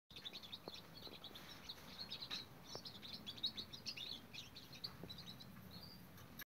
Young goldfinch mule
Young girl finch mule trying to sing 6 weeks old